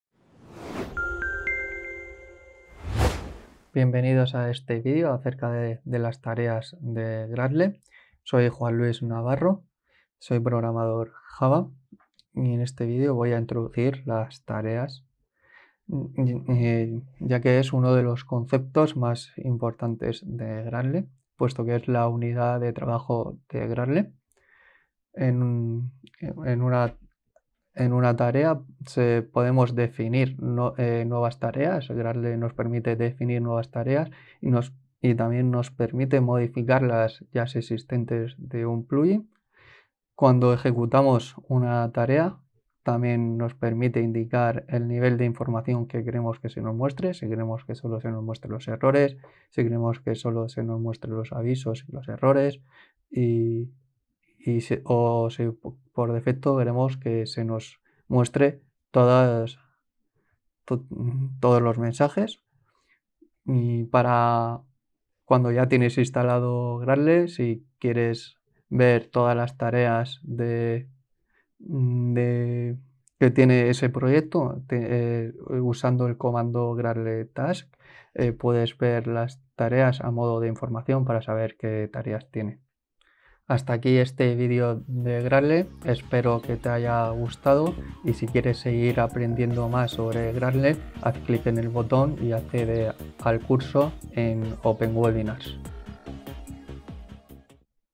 QUÉ SON LAS TAREAS DE GRADLE
Uno de los conceptos fundamentales y más importantes de Gradle son las tareas. Si quieres saber más sobre las mismas, en esta píldora te contamos qué son las tareas de Gradle y qué ventajas aportan.